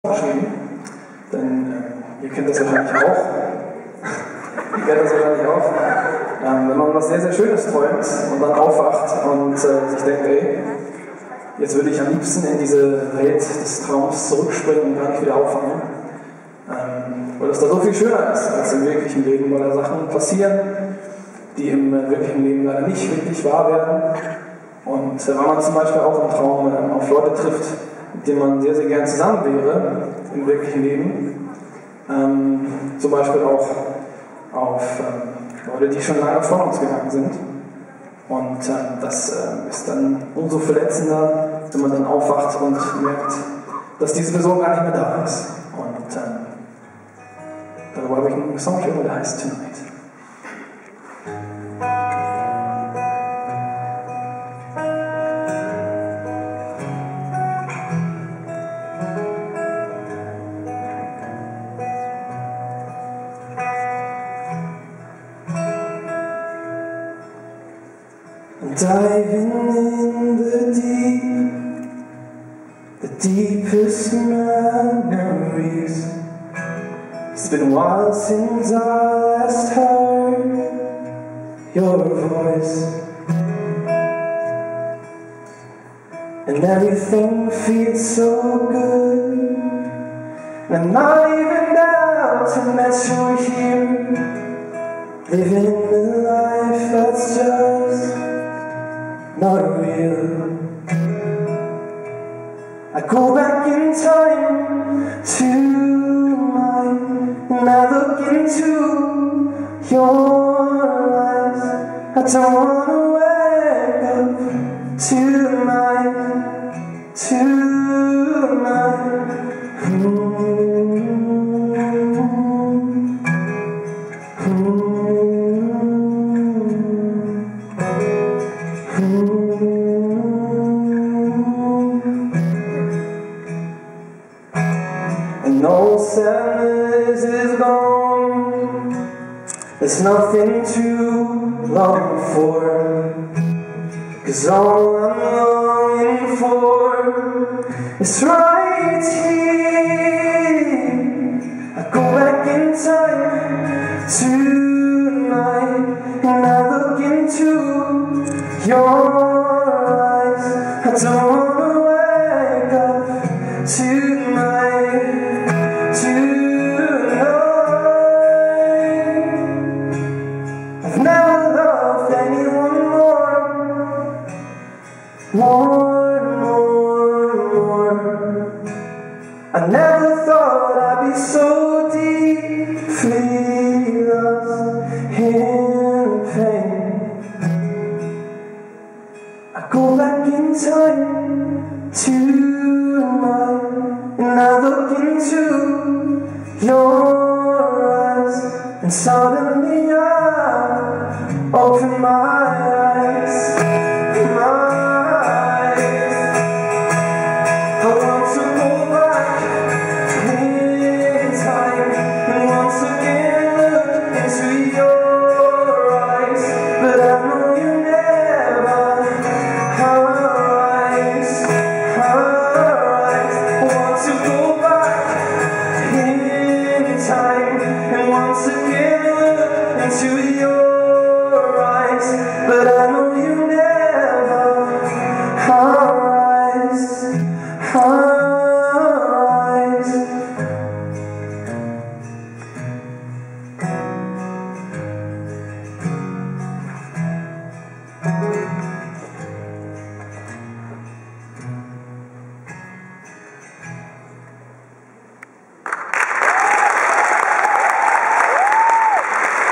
4. Michael Schulte im Capitol Mannheim
Am 1.März 2013 machte Michael Schulte im Laufe seiner Deutschland-Tour Halt im Capitol Mannheim.